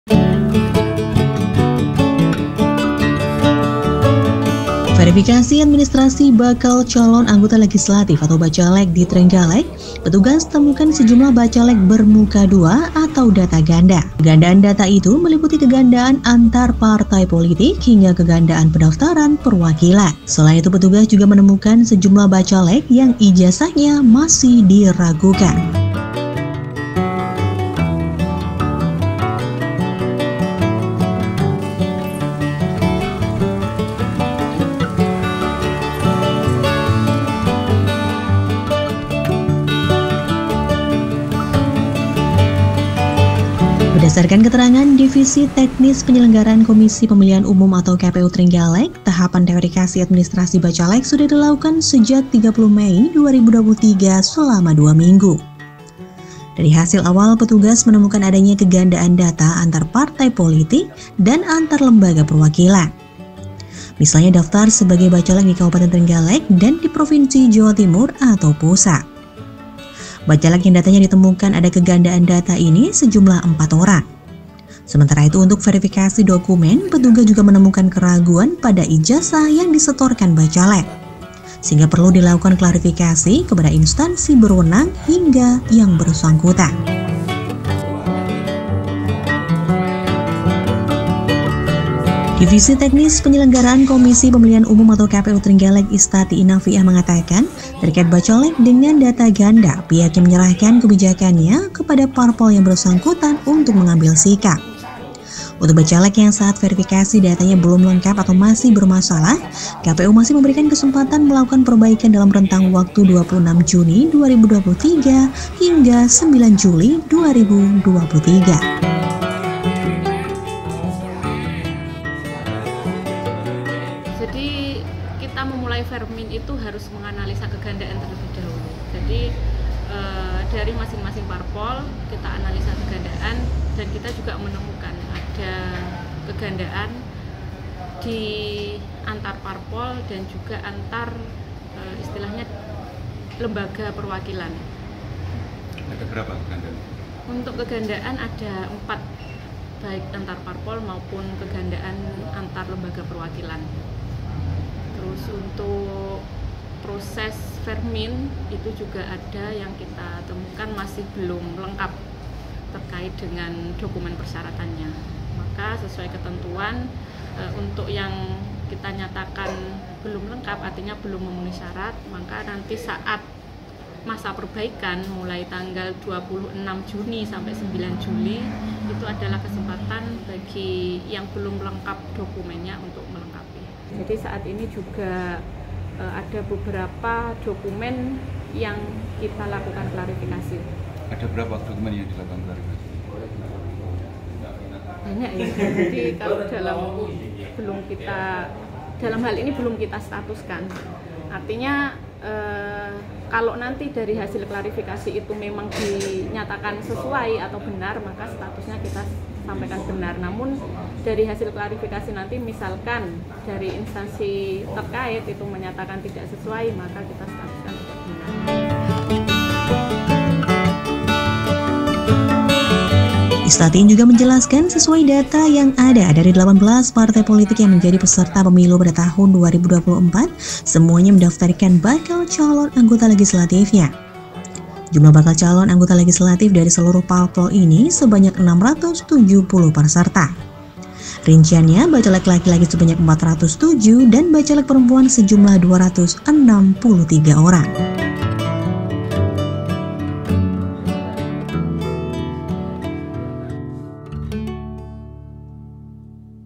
Potret Bacaleg di Trenggalek Ada Yang Bermuka Dua Hingga Ijazah Meragukan - bioztv.id
- Verifikasi administrasi bakan calon anggota legislatif (Bacaleg) di Trenggalek, petugas temukan sejumlah bacaleg bermuka dua atau data ganda. Kegandaan data itu meliputi kegandaan antar partai politik, hingga kegandaan pendaftaran perwakilan. Selain itu petugas juga temukan sejumlah bacaleg yang ijazahnya meragukan.

Berdasarkan keterangan divisi tekhnis penyelenggaraan komisi pemilihan umum (KPU) Trenggalek,  tahapan verifikasi administrasi bacaleg sudah dilakukan sejak 30 mei 2023 selama 2 minggu. Dari hasil awal petugas temukan adanya kegandaan data antar partai politik dan antar lembaga perwakilan. Misalnya, daftar sebagai bacaleg di Kabupaten Trenggalek dan di Provinsi Jawa Timur atau pusat.  Bacaleg yang datanya ditemukan ada kegandaan data ini sejumlah 4 orang. Sementara itu untuk verifikasi dokumen, petugas juga menemukan keraguan pada ijazah yang disetorkan bacaleg. Sehingga perlu dilakukan klarifikasi pada instansi berwenang hingga yang bersangkutan.

Divisi tekhnis penyelenggaraan komisi pemilihan umum (KPU) Trenggalek, Istatiin Nafiah menyampaikan, terkait bacaleg dengan data ganda, pihaknya menyerahkan kebijakannya pada parpol yang bersangkutan untuk mengambil sikap. Untuk bacaleg yang saat verifikasi datanya belum lengkap atau masih bermasalah, KPU masih memberikan kesempatan melakukan perbaikan dalam rentang waktu 26 Juni 2023 hingga 9 Juli 2023.

Istatiin juga menjelaskan, sesuai data yang ada, dari 18 partai politik yang menjadi peserta pemilu pada Tahun 2024, semuanya mendaftarkan bakal calon anggota legislatifnya (Bacaleg) di Kabupaten Trenggalek.   Jumlah bakal calon anggota legislatif dari seluruh parpol ini sebanyak 670 peserta.  Rinciannya bacaleg laki laki sebanyak 407 dan bacaleg  perempuan sejumlah 263 orang.